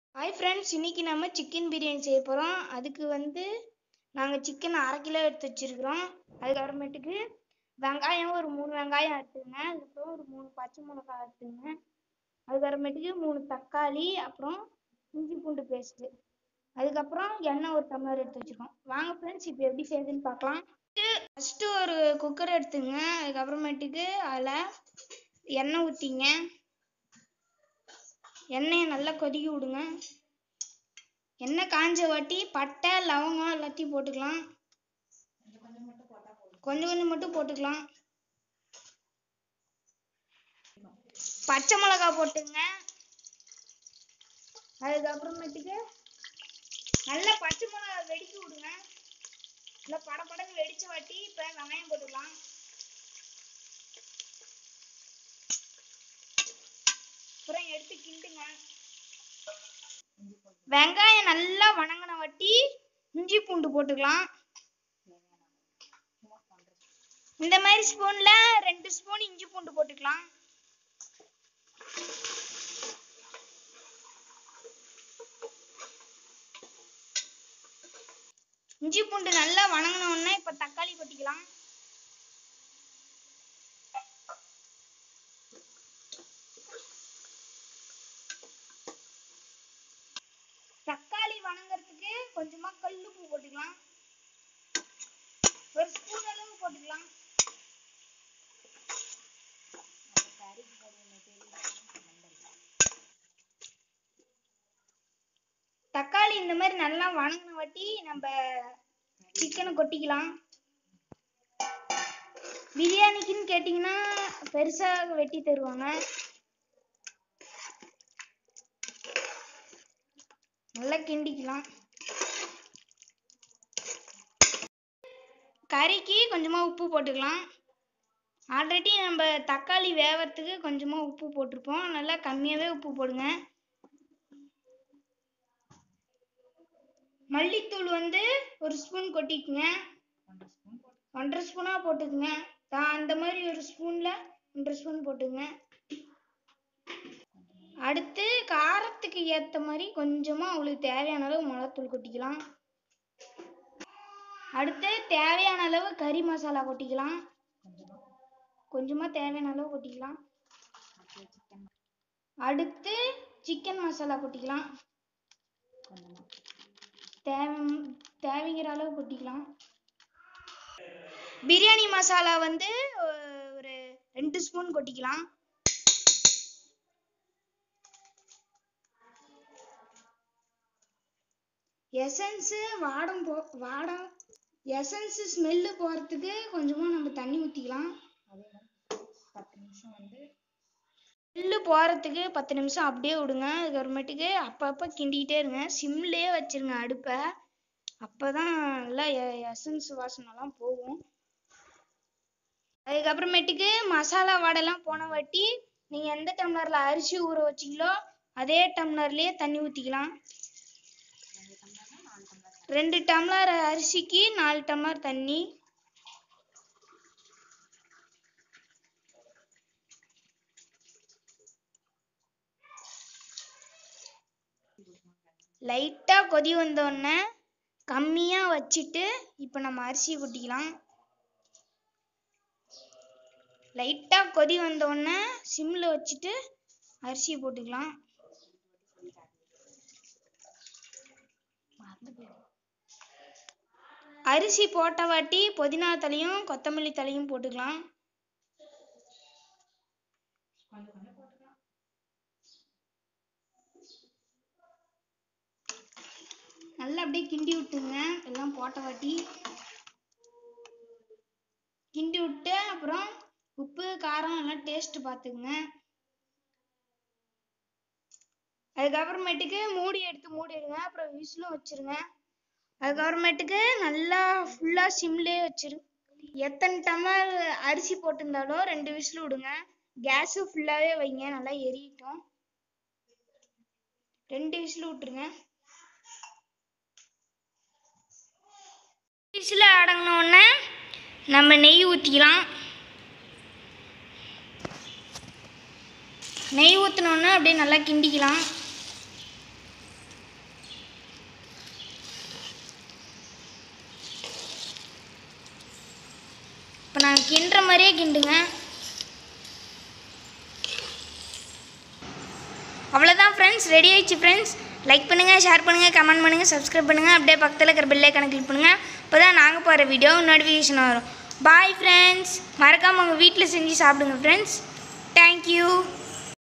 How to Make Chicken Biryani at Home in Tamil / Chicken Biryani recipe at Home / Harikrishna ldeas
How to Make Egg Maggi at Home in Tamil